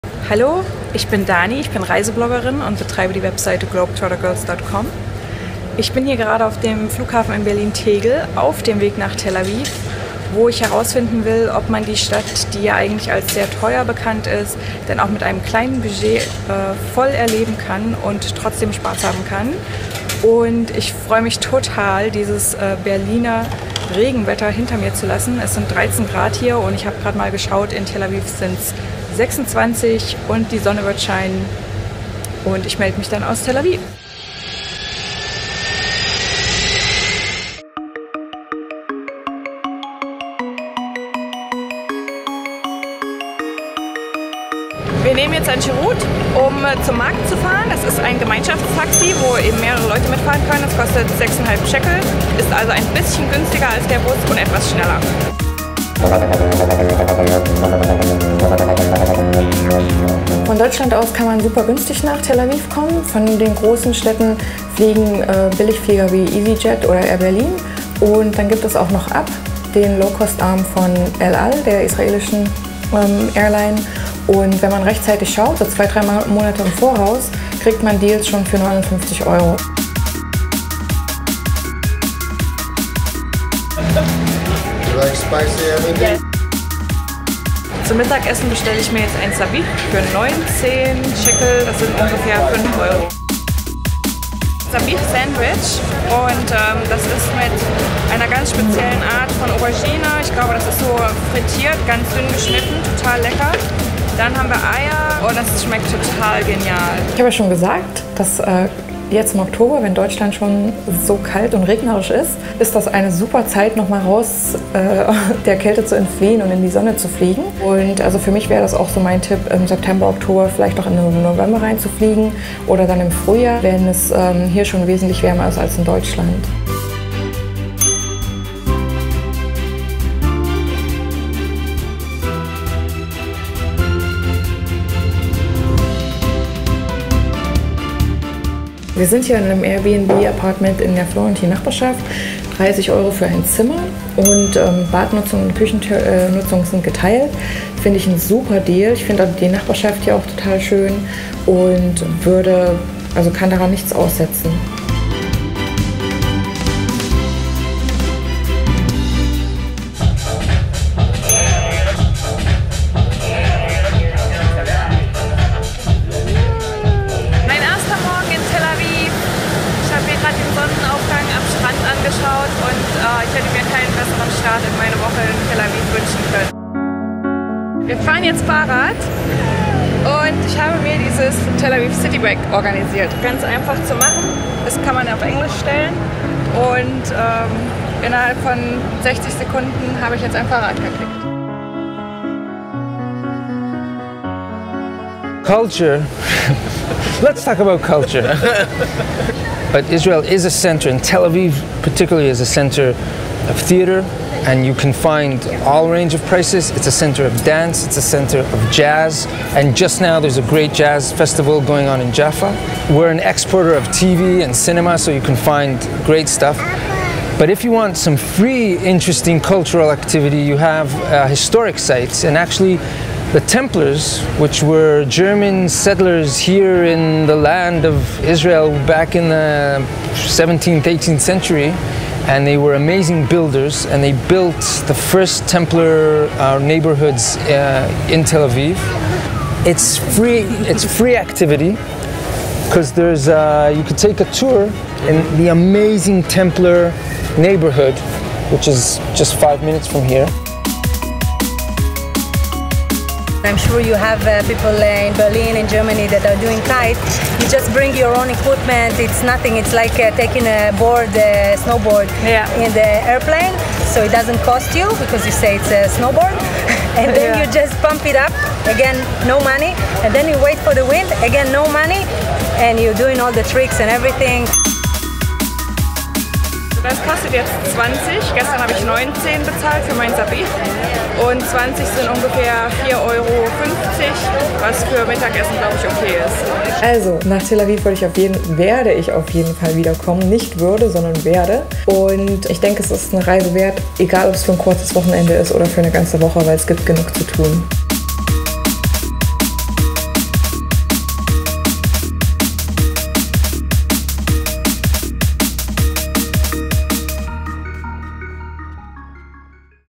Kann ich Tel Aviv für NUR 69 Euro pro Tag bereisen?
Die Herausforderung: Tel Aviv mit 69 Euro pro Tag!